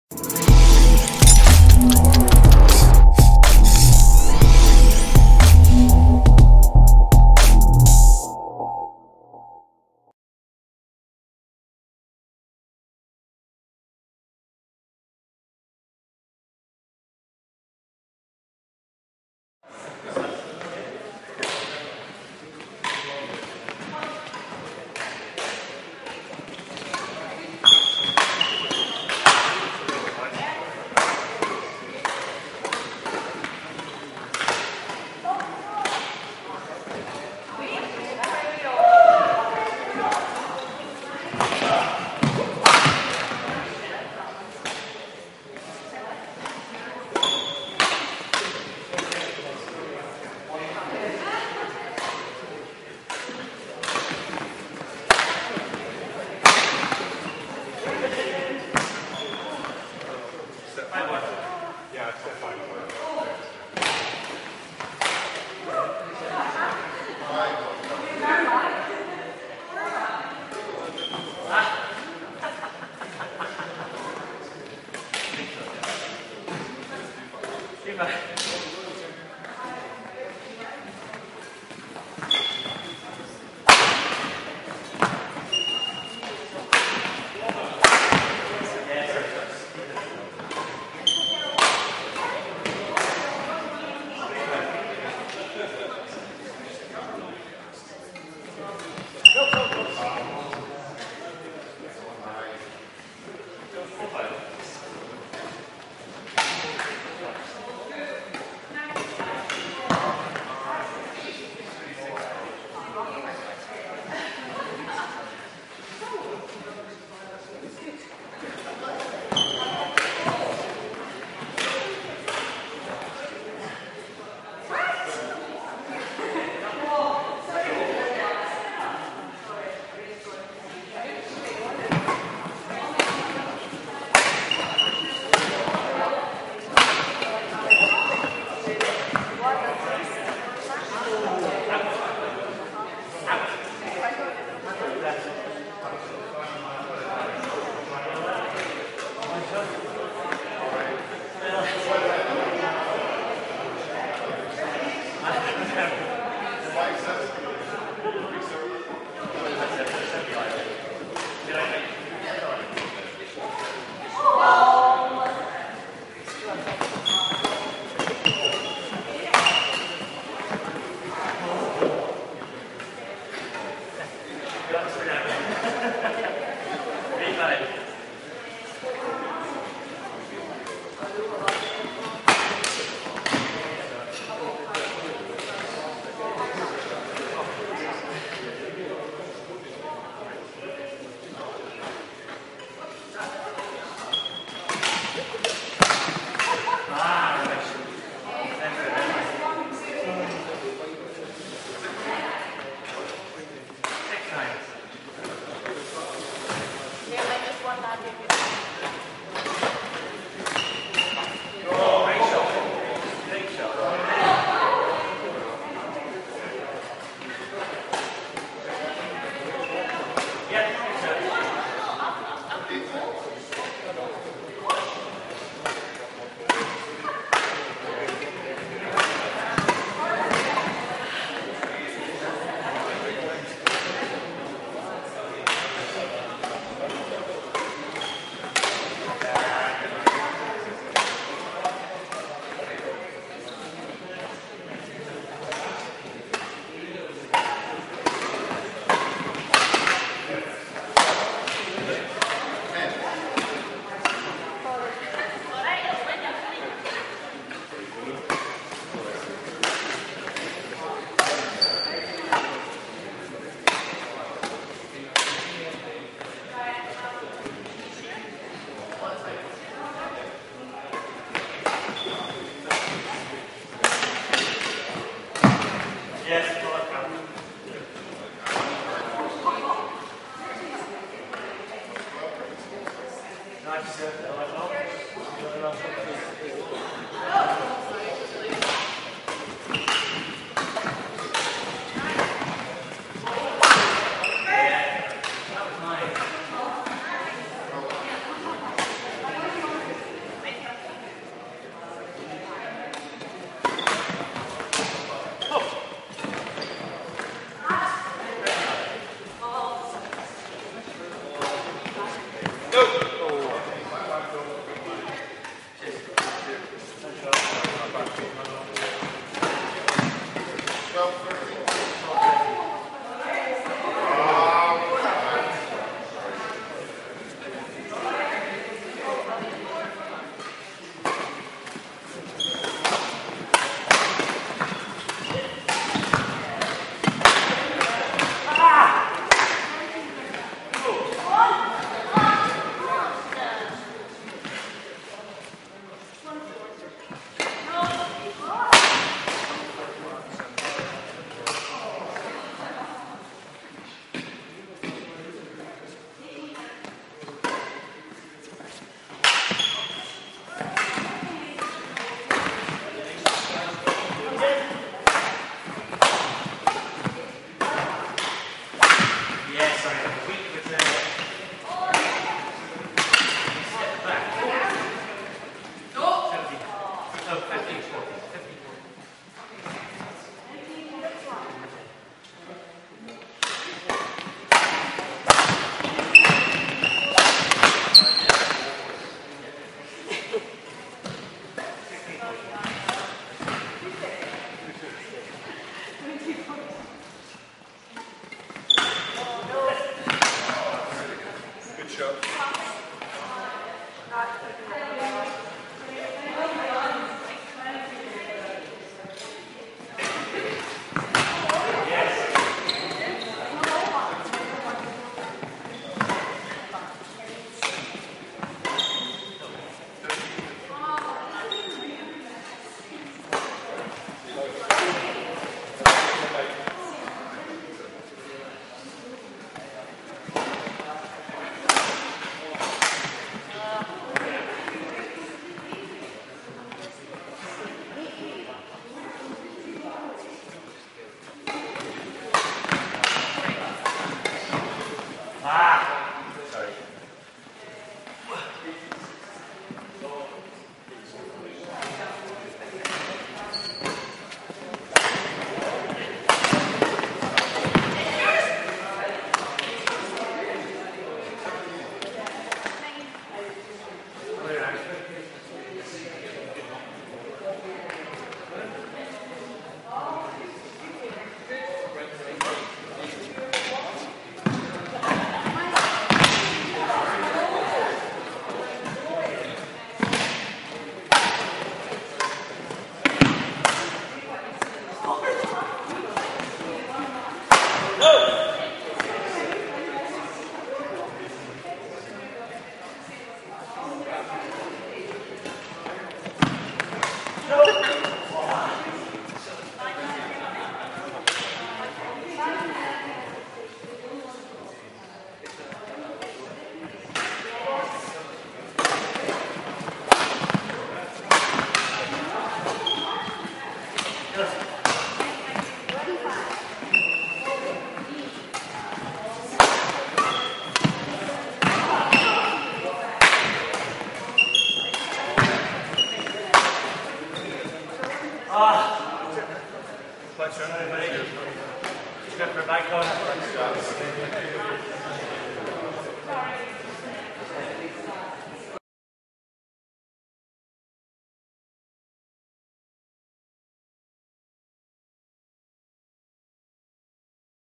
Borders Badminton Group - Open Tournament - February, 2023.
Gents doubles game played at the Borders Open Tournament at Earlston High School.